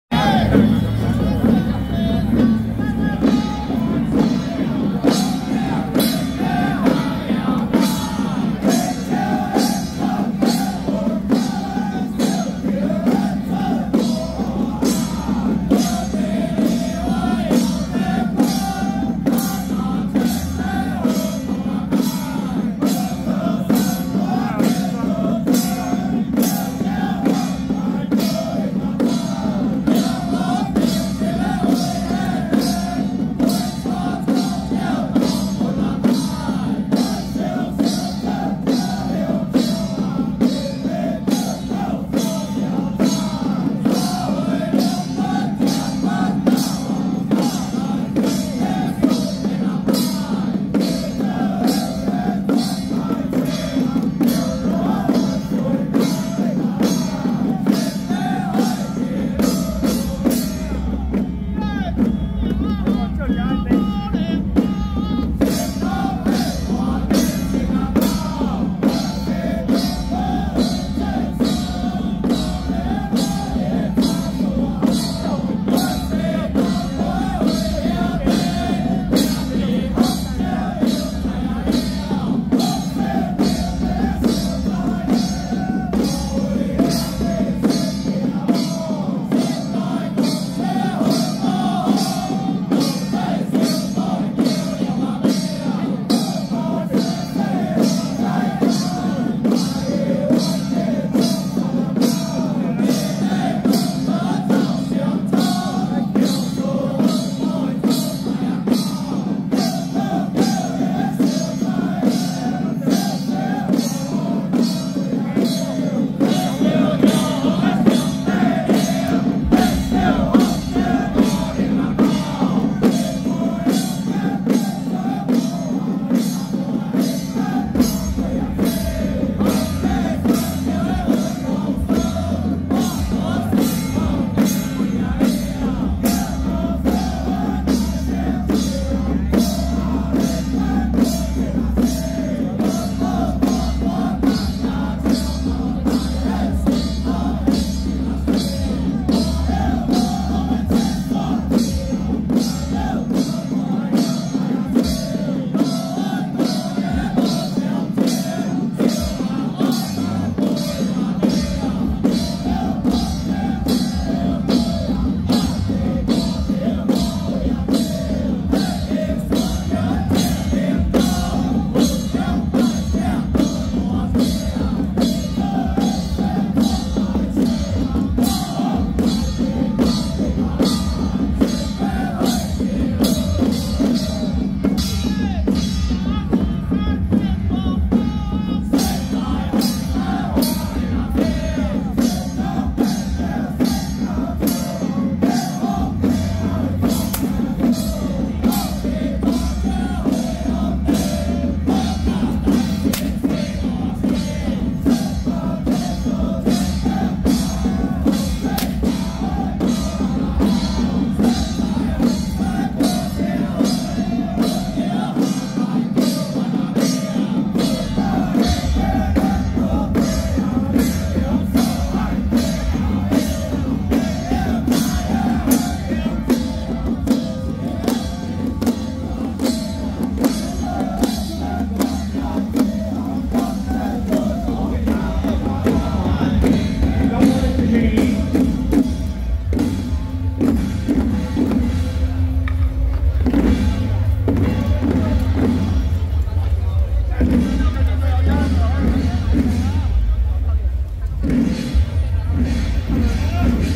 【五股拱聖堂-小法團】~113新莊地藏庵恭祝文武大眾老爺聖誕正日遶境(中港區)
五股拱聖堂-小法團